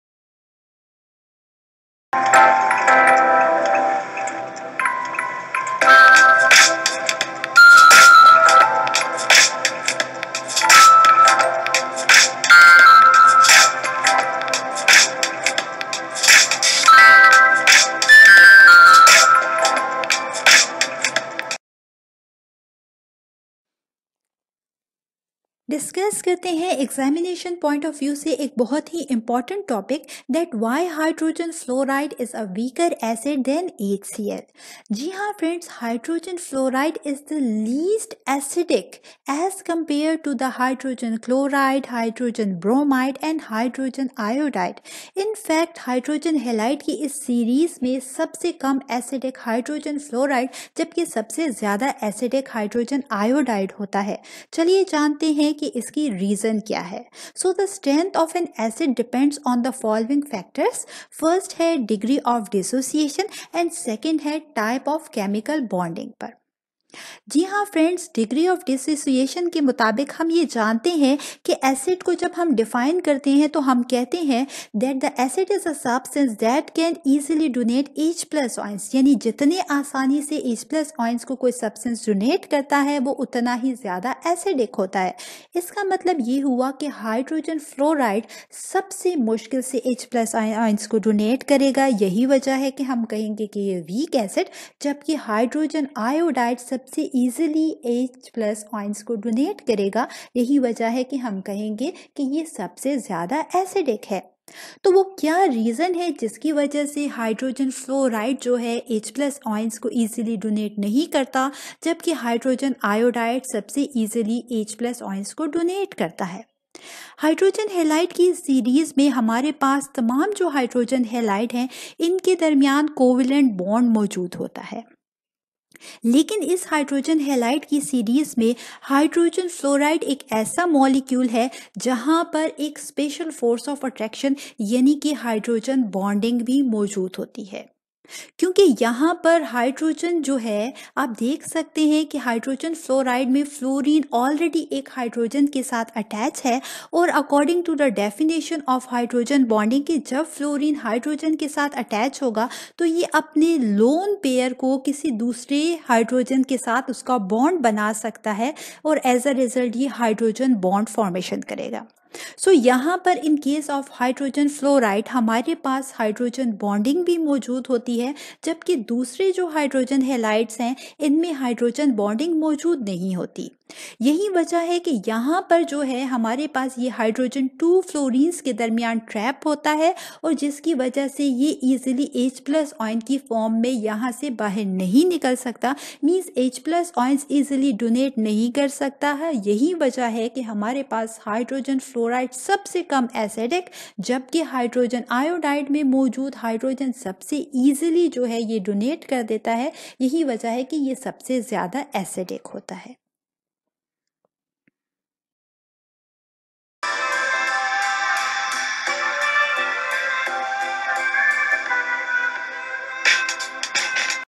Why hydrogen fluoride is a weaker acid than hydrochloric acid | Find acidic order of HF,HCl,HBr ,HI
Why hydrogen fluoride is a weaker acid than hydrochloric acid ?
Increasing acidic order is HF,HCl,HBr ,HI
The strength of an acid depends on :
1.Degree of dissociation
2.Types of bonding
Due to hydrogen bonding, HF is least acidic than HCl, HBr & HI .The H- atom in HF is entrapped b/w two fluorine atoms and not easily available as a proton (H+).